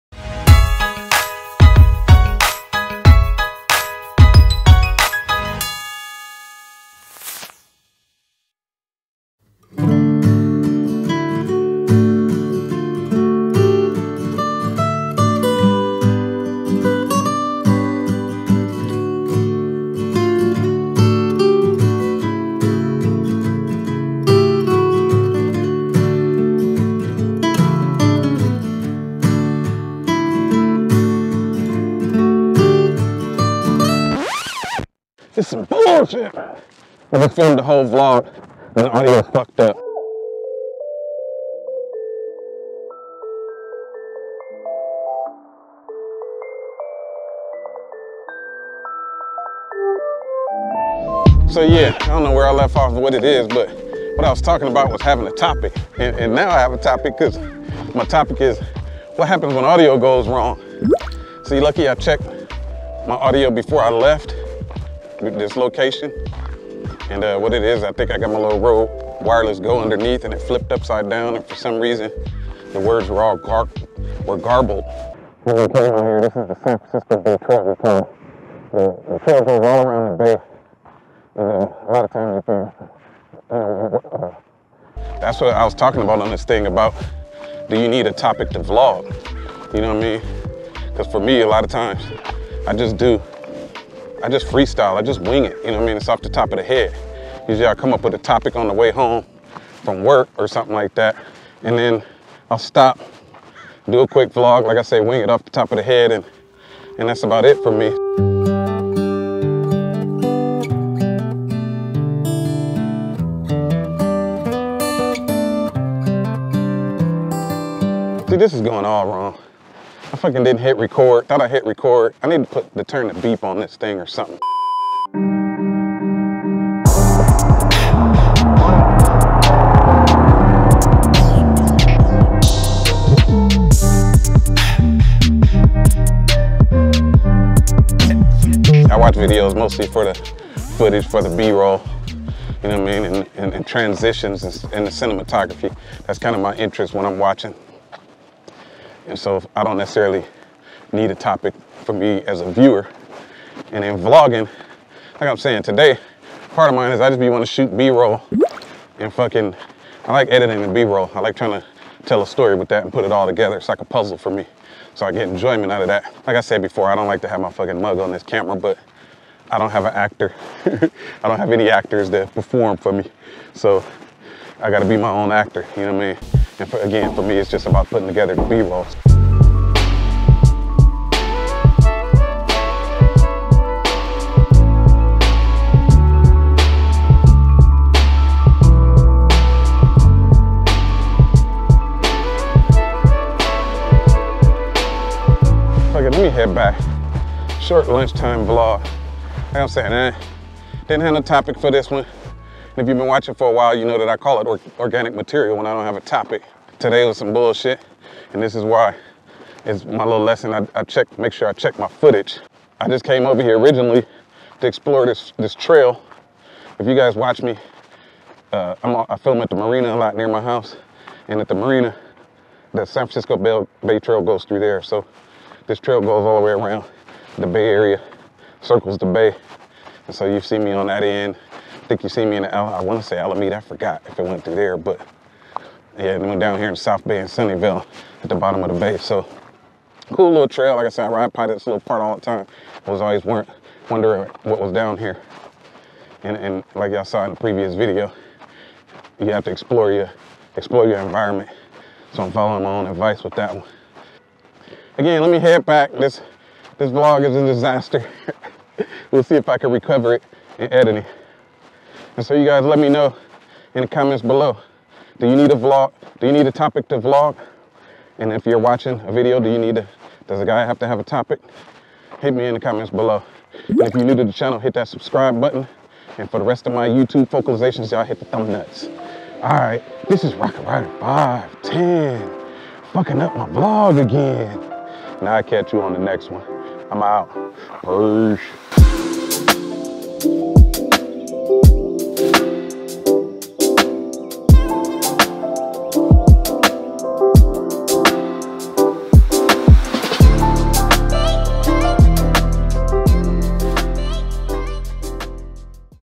Disaster Vlog - Organic Material
This vlog was a filming disaster!  I had a few issues, but I'm proud of the way I worked it out in the edit.  My favorite vids are always the ones that present a challenge.  So, even though I'm rambling and don't have a topic, I enjoy the finished product that much more.

Don't use any of these?